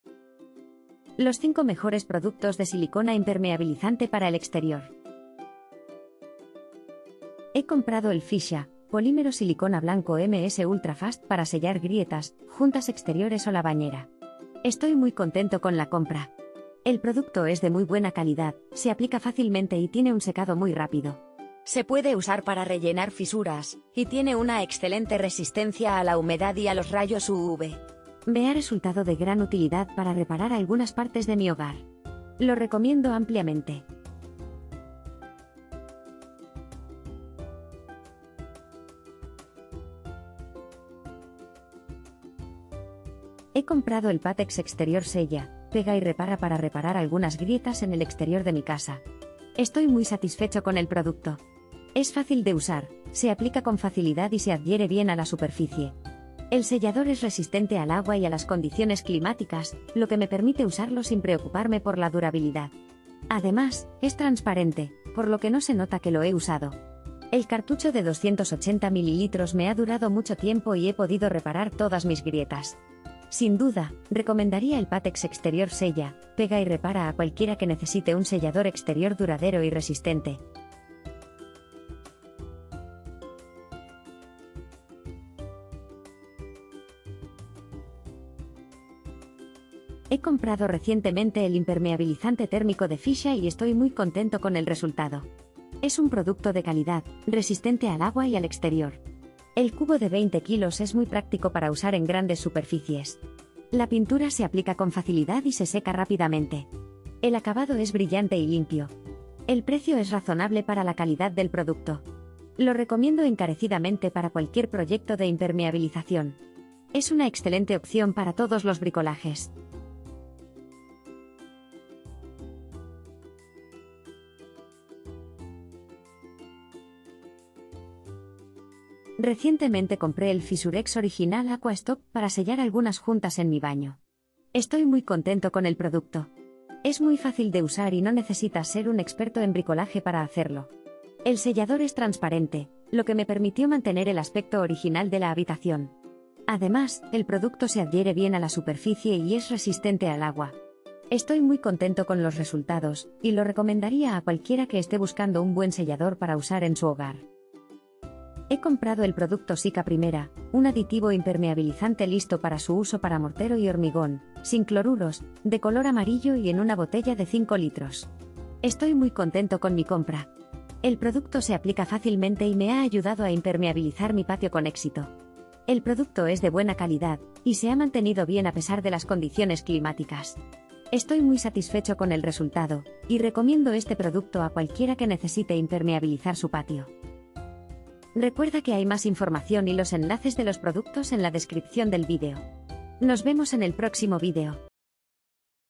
【SILICONA IMPERMEABILIZANTE EXTERIOR】
✋  Aquí están los 5 mejores productos de silicona impermeabilizante para el exterior disponibles en el mercado.

🔽🔽🔽 ENLACES DE COMPRA SILICONA IMPERMEABILIZANTE EXTERIOR 🔽🔽🔽

1) fischer | polímero silicona blanco MS Ultra fast. Sellador adhesivo de secado muy rápido para grietas, juntas exteriores o bañera, relleno fisuras. (290 ml)

2) Pattex Exterior Sella, Pega & Repara, sellador exterior duradero, polímero transparente resistente a las condiciones climáticas, cubrejuntas pintable, 1 cartucho x 280 ml

3) fischer - Pintura impermeabilizante térmico (cubo 20kg) resistente al agua y exteriores

4) FISUREX Original Aqua Stop 60ml- Sellador Juntas Transparente Acrilico Pintable - Penetra Pega Sella - silicona transparente liquida impermeabilizante - Pegamento antifugas agua sacar humedad de pared

5) Sika 1a, Aditivo impermeabilizante, listo para su uso, para mortero y hormigón, sin cloruros, Amarillo, 5L

00:00 - Intro
00:06 - 1) fischer | polímero silicona blanco MS Ultra fast.
00:42 - 2) Pattex Exterior Sella, Pega & Repara, sellador exterior...
01:28 - 3) fischer - Pintura impermeabilizante térmico (cubo 20kg)...
02:08 - 4) FISUREX Original Aqua Stop 60ml- Sellador Juntas...
02:37 - 5) Sika 1a, Aditivo impermeabilizante, listo para su uso,...
03:06 - Outro